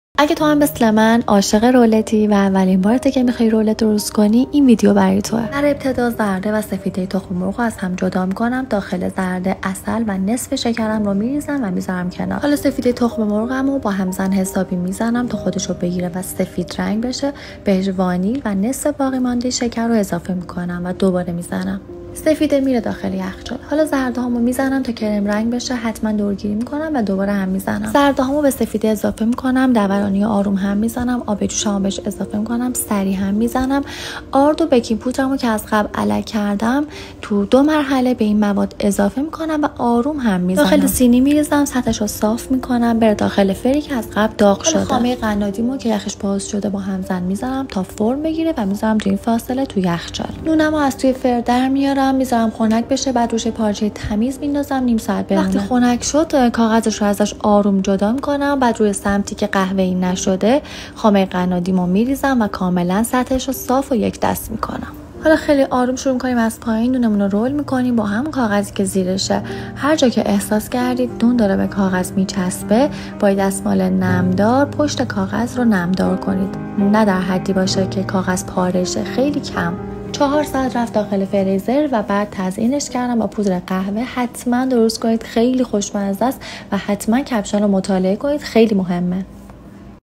Be sure to try the delicious roll😍این رولت محشره😍 حتماً امتحان کنید 👌food recipe dessert cake#
4 eggs, 10 grams of vanilla, 80 grams of sugar, 3 spoons of boiling water, honey, 1 spoon of baking powder, half a spoon of flour, 100 grams of cream, 400 grams of preheated oven temperature, 200 degrees, and 10 minutes of baking.
4عدد تخم مرغ 10گرم وانیل 80گرم شکر 3قاشق اب جوش عسل 1قاسق بیکین پودر نصف قاشق ارد100گرم خامه 400گرم دمای فر از قبل گرم شده 200 با 10دقیقه پخت